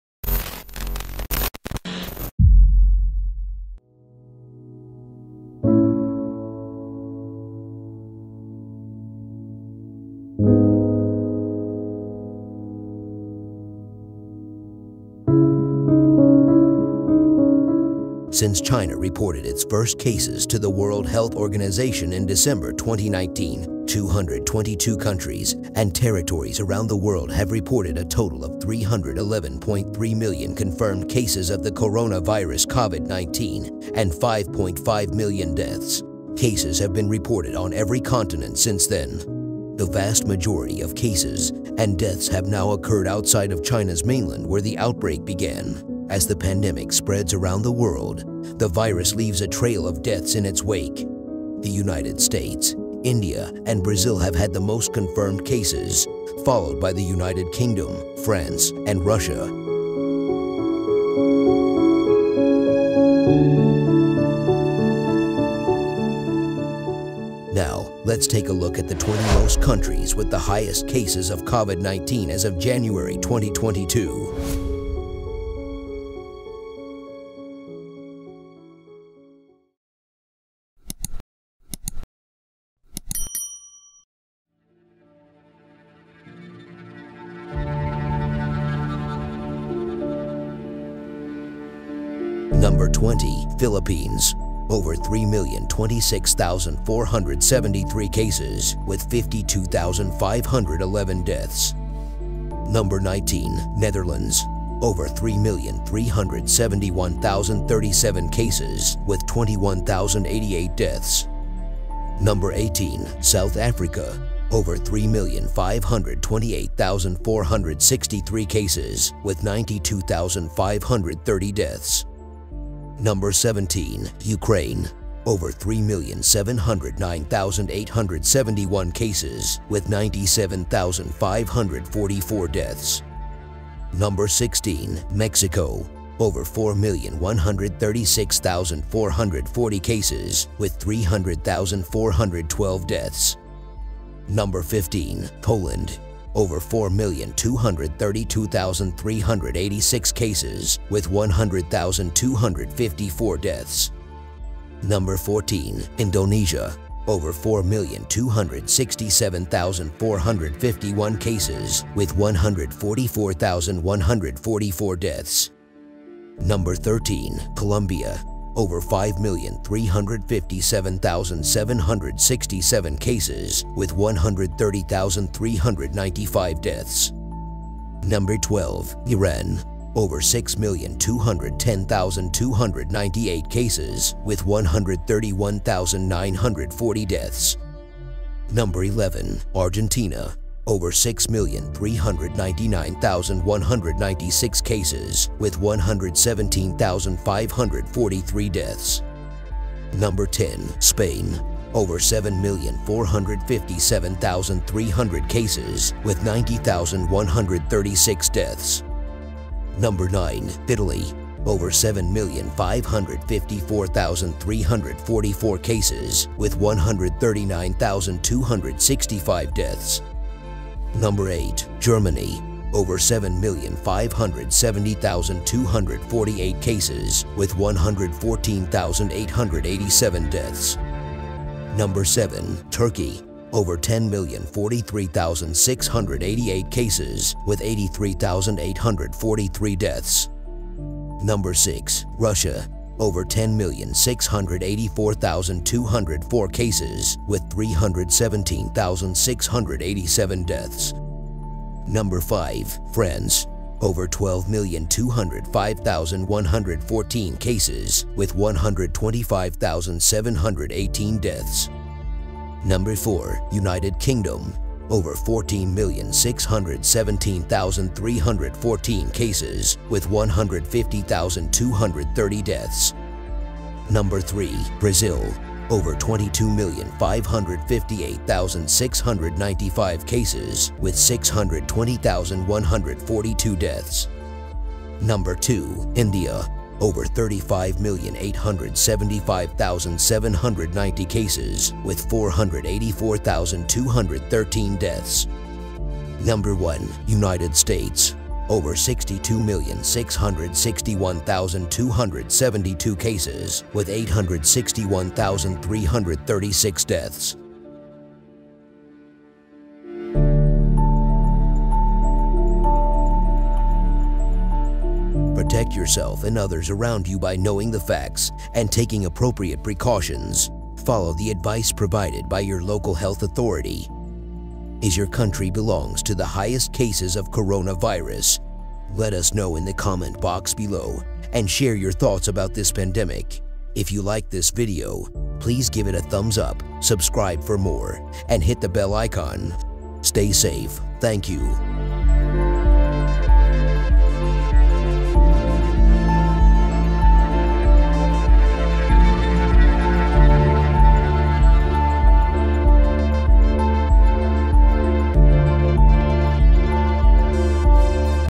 20 Most Countries with the HIGHEST CASES of COVID-19 | 2022
Since China reported its first cases to the World Health Organization in December 2019, 222 countries and territories around the world have reported a total of 311.3 million confirmed cases of the coronavirus COVID-19 and 5.5 million deaths. Cases have been reported on every continent since then. The vast majority of cases and deaths have now occurred outside of China's mainland, where the outbreak began. As the pandemic spreads around the world, the virus leaves a trail of deaths in its wake. The United States, India, and Brazil have had the most confirmed cases, followed by the United Kingdom, France, and Russia.

20 Most Countries with the HIGHEST CASES of COVID-19 | 2022

Top Club2181 provides you with the most informative and interesting top videos in the world!